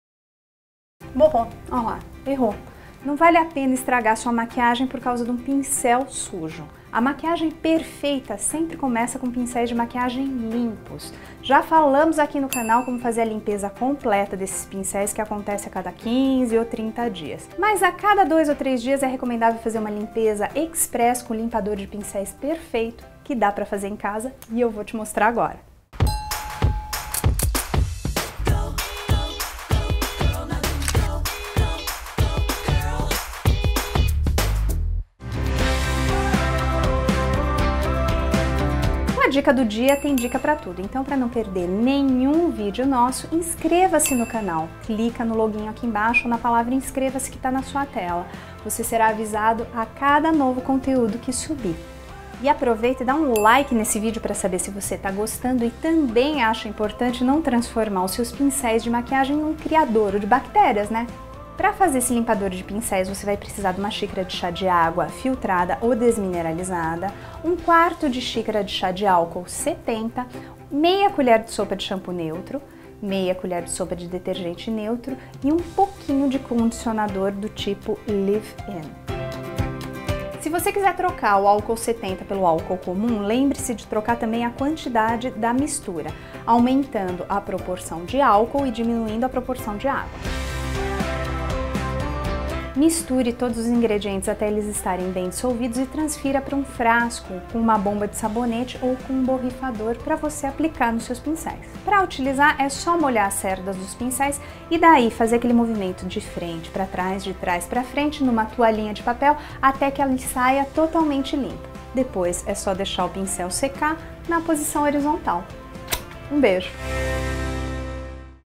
COMO FAZER LIMPADOR DE PINCEL DIY | FLÁVIA FERRARI
Anote essa receita para fazer limpador de pincel de maquiagem em casa (DIY - Do It Yourself). Esta solução também atua como um higienizador de pincel. Muito fácil de fazer!!